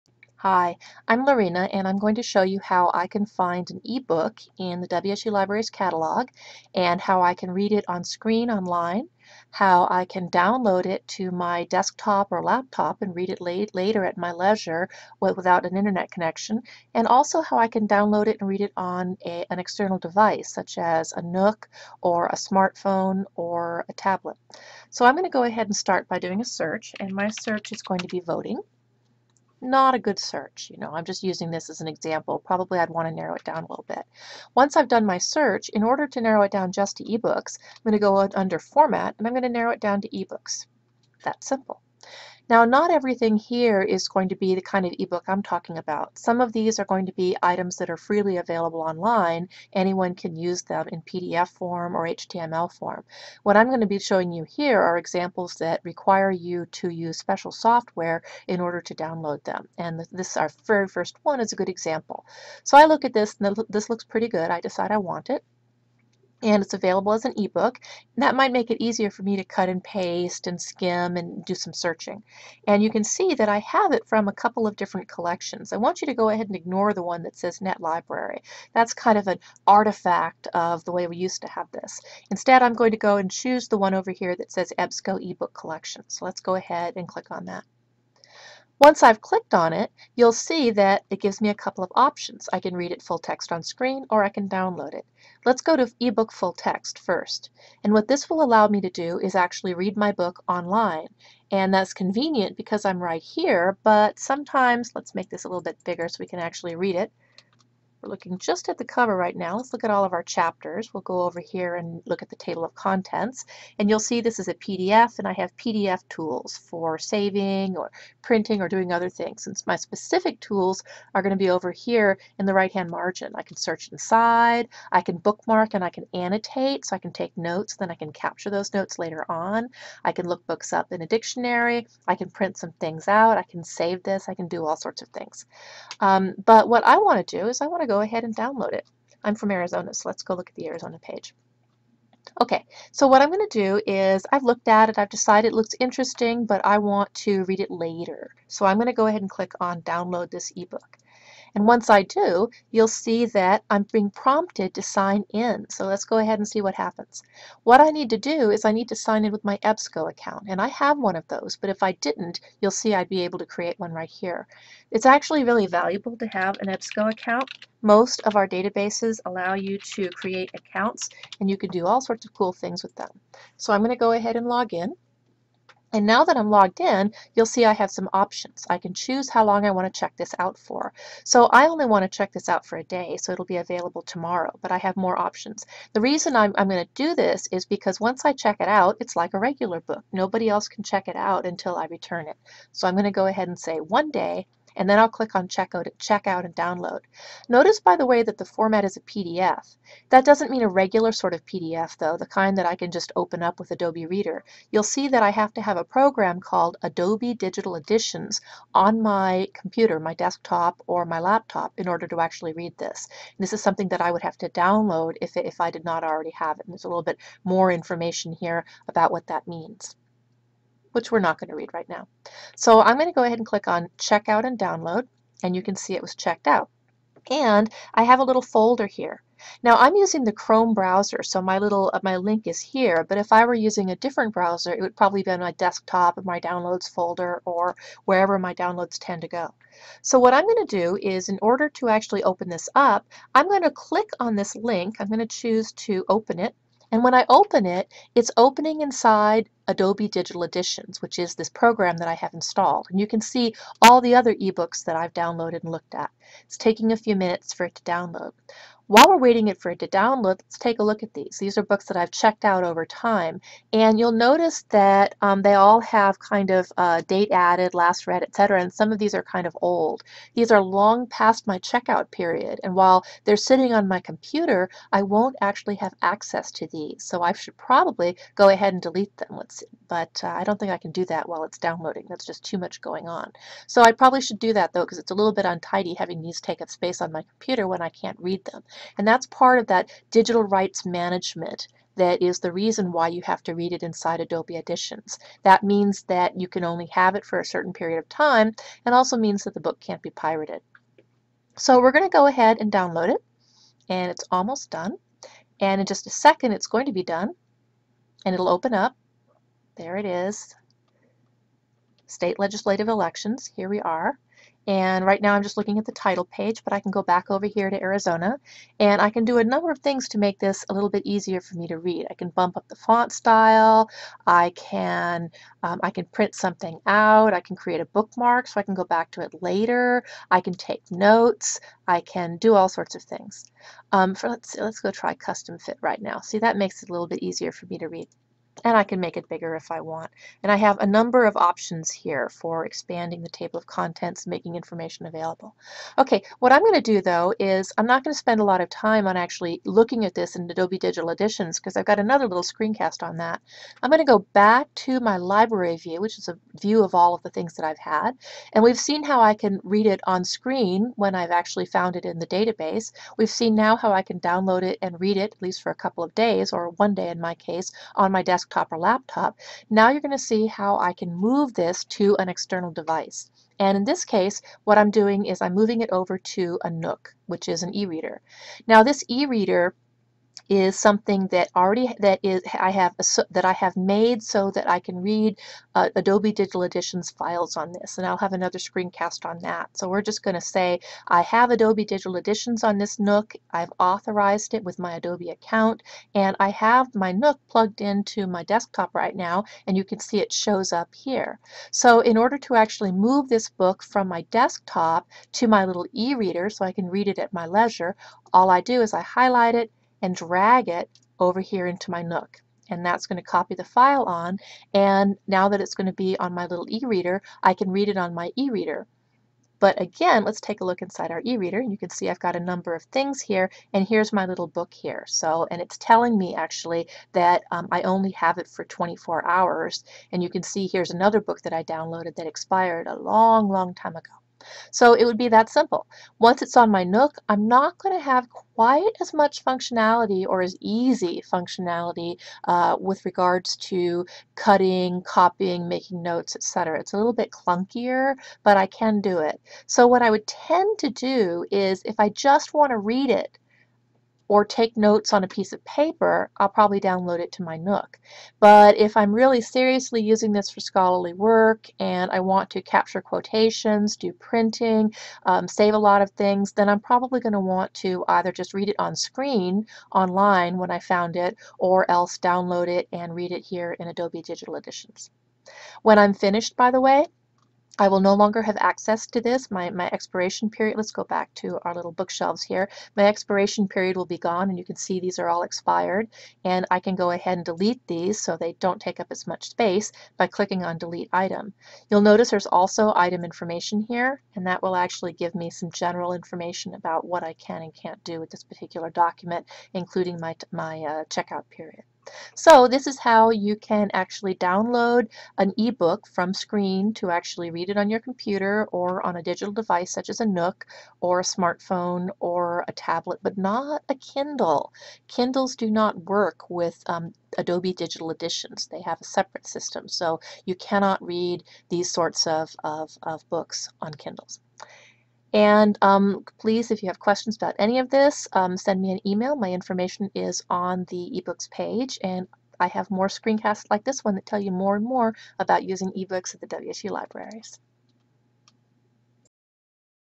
Reading and Downloading EBSCO eBook Collection ebooks
This 9-minute screencast demonstrated how to locate an ebook in the WSU Libraries catalog, read it online, download it to your desk/laptop using the required Adobe Digital Editions software, and download it to a nook ereader. If your ebook is from EBL or ebrary the process will be very similar. Note 1: In this screencast I have already downloaded and authorized Adobe Digital Editions (ADE) on my desktop computer and on my nook ereader. Note 2: This is an example of an ebook that can only be read on an ADE-compatible ereader (such as a nook or Sony ereader) or a tablet or smartphone using ADE-compliant apps such as BlueFire, Aldiko, or a vendor-provided app (ebrary has one for iOS and Android). You cannot read ADE books on a Kindle! Kindle Fire? I'll be checking that out.